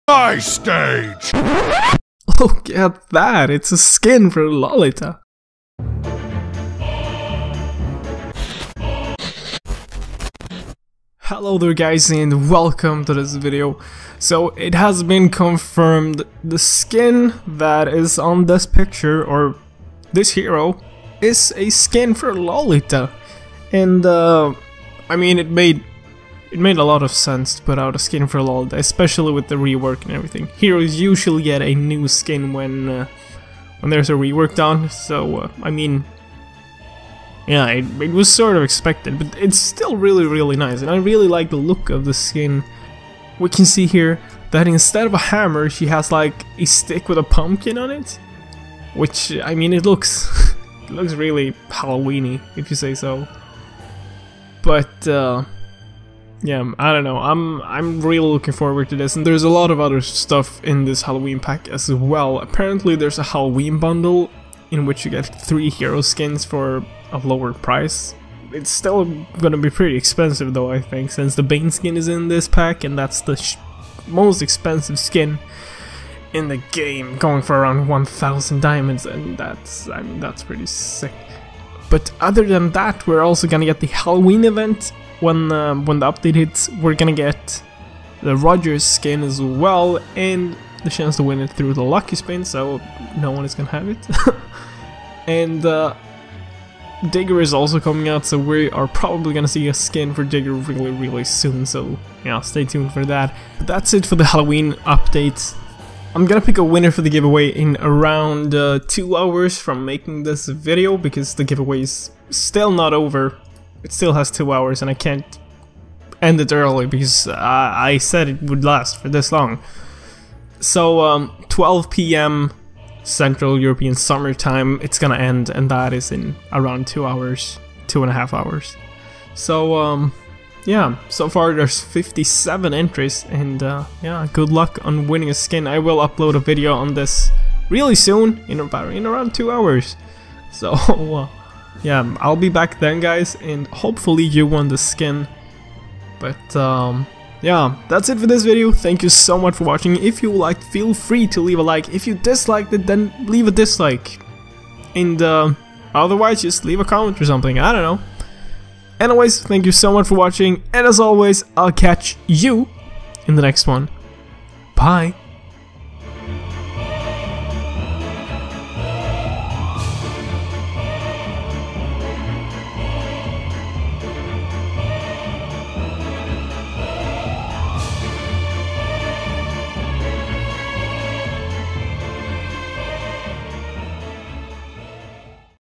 It's a skin for Lolita! (Trickster's eve Event!) Mobile Legends!
So it's now confirmed guys, the skin that was on the loading screen for mobile legends is a Lolita skin! I'm really excited for this and I hope you are aswell!

Thanks! :D

TjabbaSquad Unite!